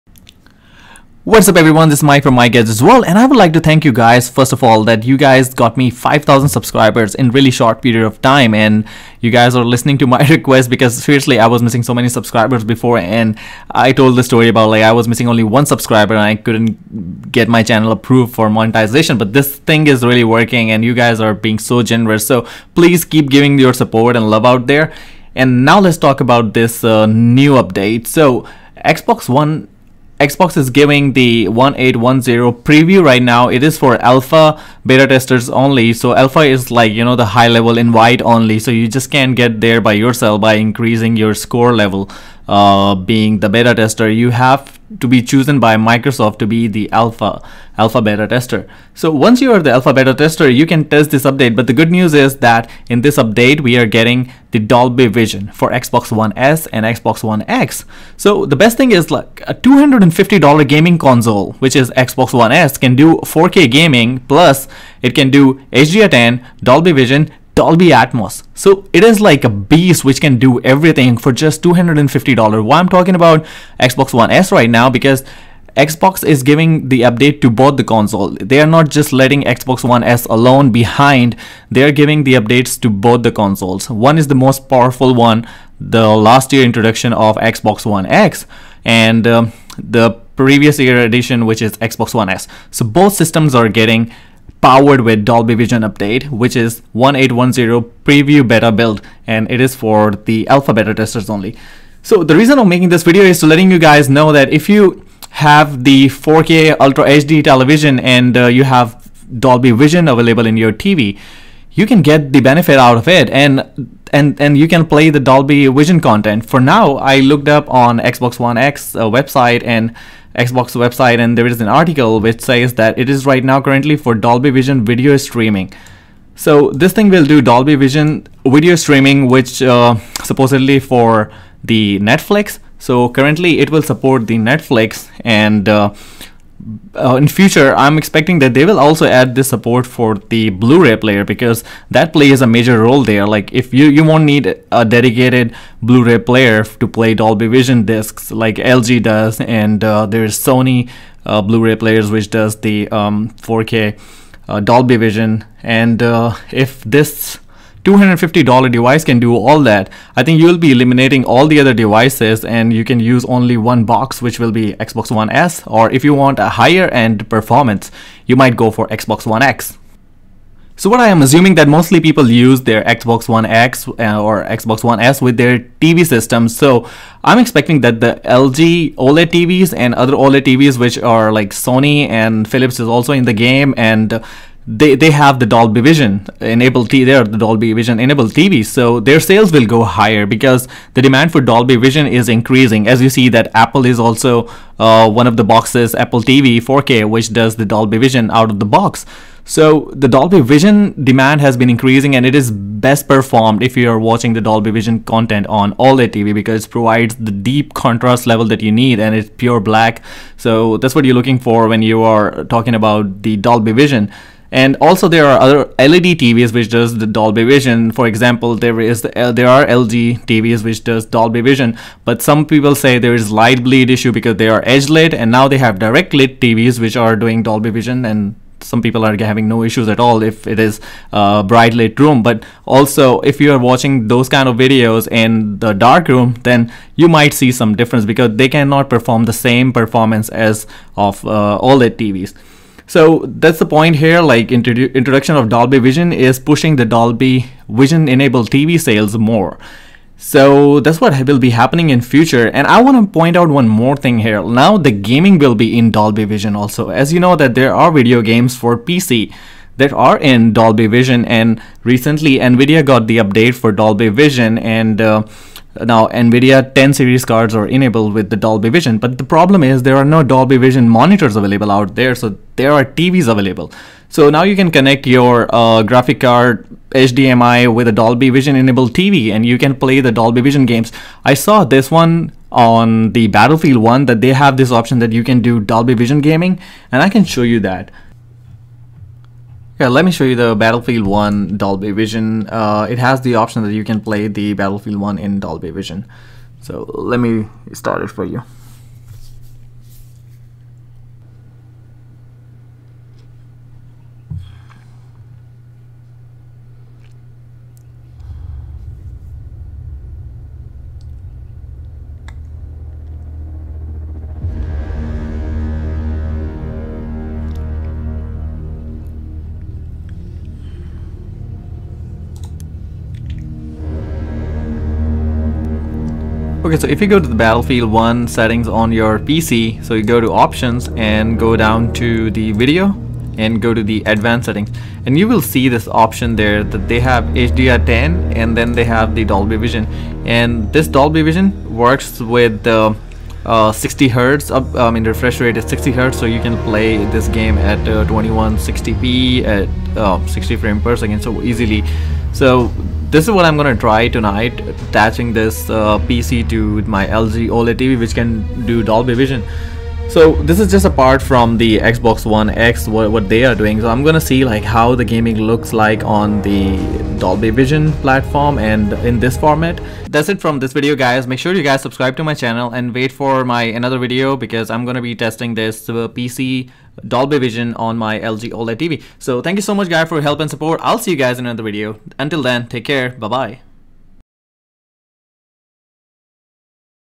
Xbox One X and S Dolby Vision Update
Microsoft is preparing to add Dolby Vision support to its Xbox One S and Xbox One X consoles. The addition will mean Microsoft’s consoles join the Apple TV 4K and Google’s Chromecast Ultra as the only streaming devices that currently support both Dolby Vision and Dolby Atmos. Microsoft is testing out its Dolby Vision support with Xbox Insiders right now (Alpha Testers only), and it appears to be limited to streaming apps for now. Netflix is one of the first apps to be updated with Dolby Vision for the Xbox One S and Xbox One X, and it’s not clear if Microsoft plans to extend this to Blu-ray discs too.

Dolby Vision is widely considered to be more superior to the current HDR10 support found on the Xbox One. Dolby Vision adjusts the brightness and color of scenes with metadata, and supports up to a 12-bit color depth and higher peak brightness targets than HDR10. While you won’t necessarily see an immediate difference on current TV sets, Dolby Vision support (alongside HDR10) on the Xbox One makes it more future proof in the current HDR standards war. Microsoft’s main rival console, Sony’s PlayStation 4, still doesn’t fully support Dolby Atmos or Dolby Vision.

Source: TheVerge.com